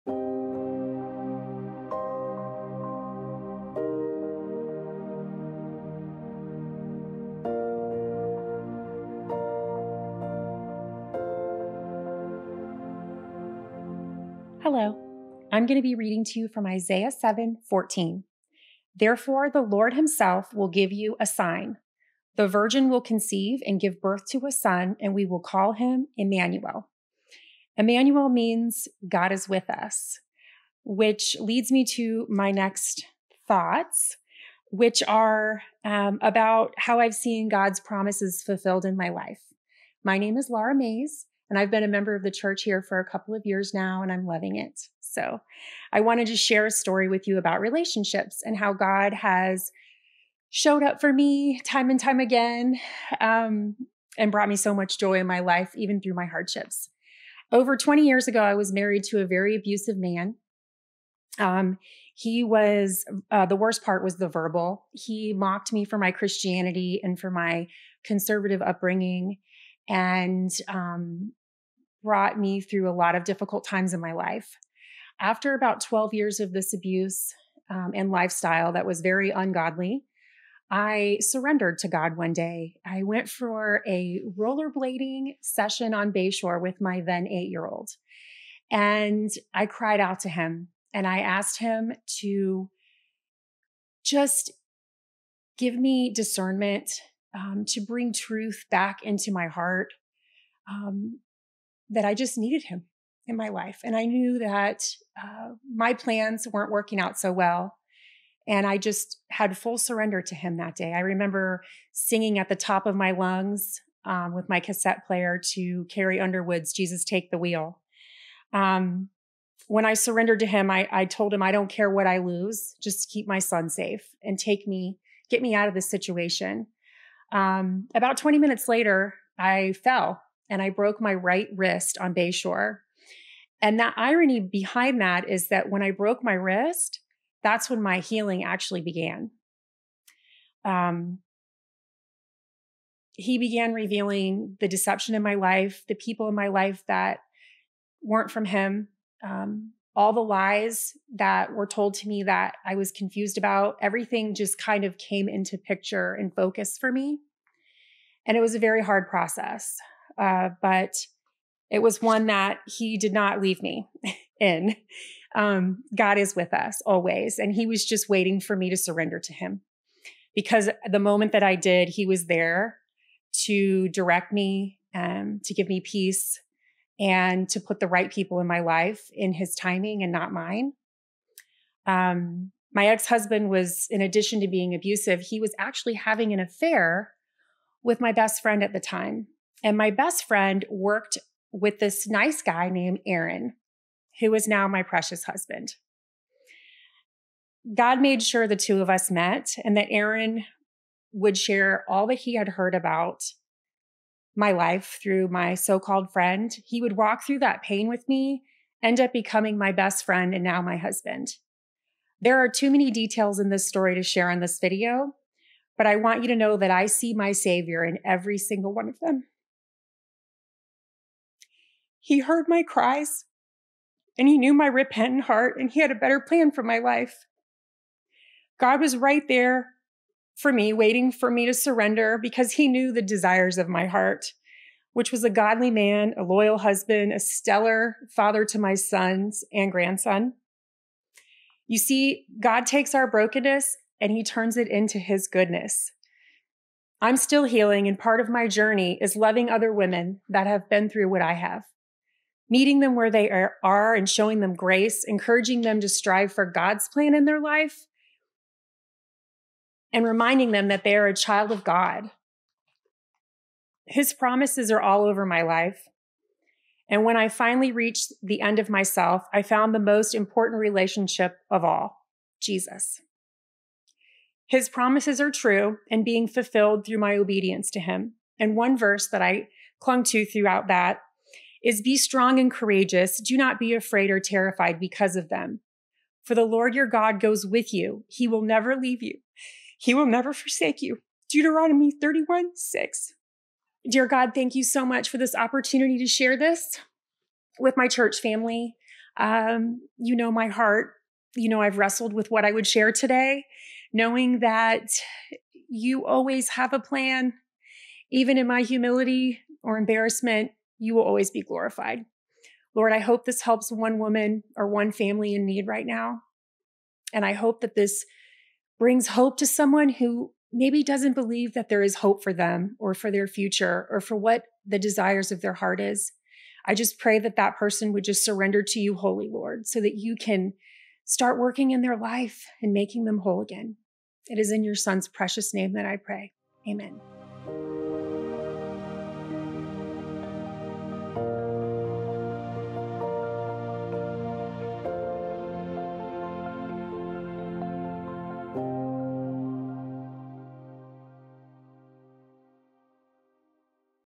Prophetic Fulfillment: The Virgin Birth | Day 12: An Advent Reading Plan
In today's video, Lara Mays reads through Isaiah 7:14, underlining God's faithfulness in the prophecy of a virgin birth.